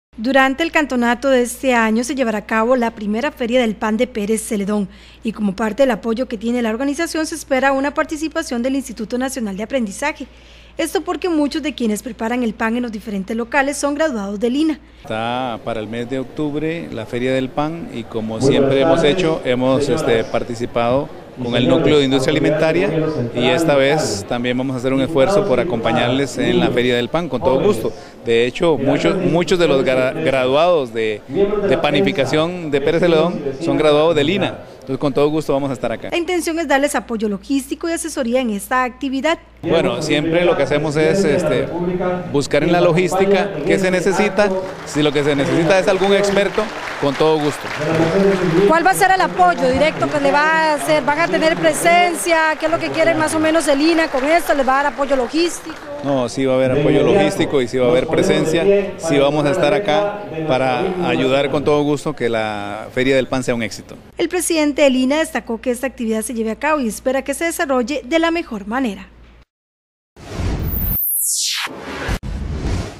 INA brindará apoyo a la I Feria del Pan en Pérez Zeledón
. Durante el cantonato de este año, se llevará a cabo la primera Feria del Pan de Pérez Zeledón y como parte del apoyo que tiene la organización, se espera una participación del Instituto Nacional de Aprendizaje.